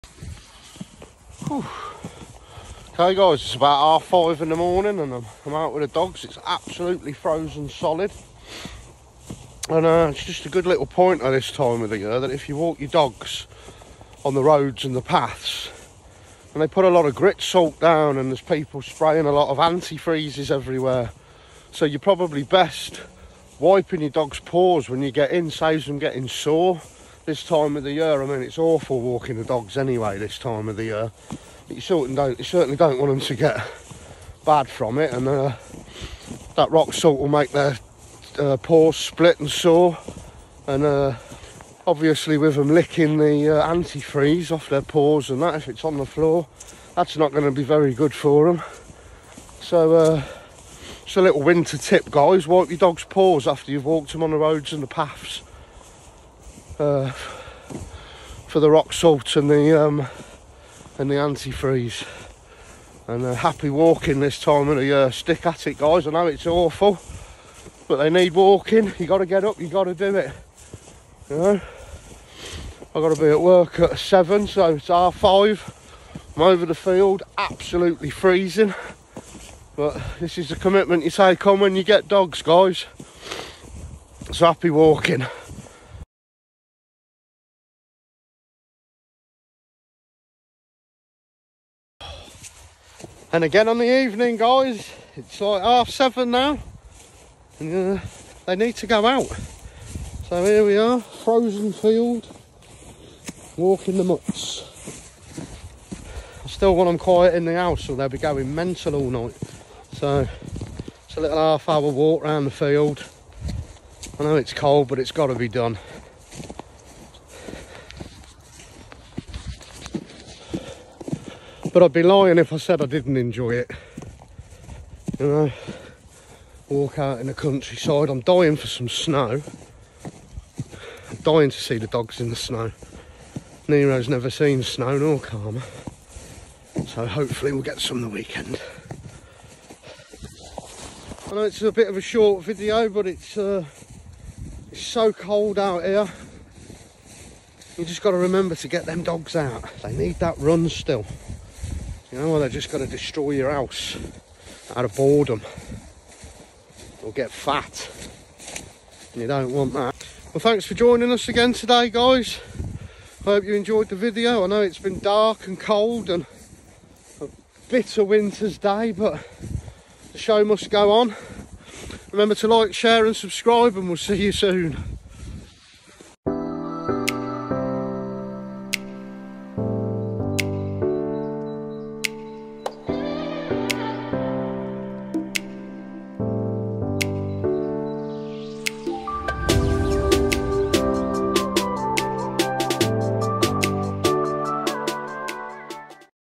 A must know tip for winter dog walking
Your dog still needs to go out in the winter time but you should always wipe,their paws afterwards#canecorso